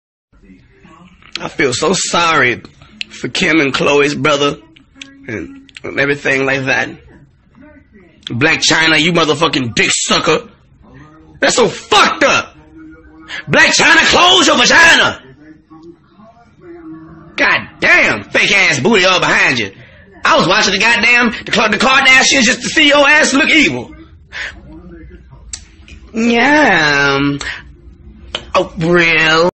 Orlando Brown Has Words For Blac Chyna After Seeing What Rob Kardashian Is Going Through.
Orlando Brown Has Words For Blac Chyna After Seeing What Rob Kardashian Is Going Through. What do you think?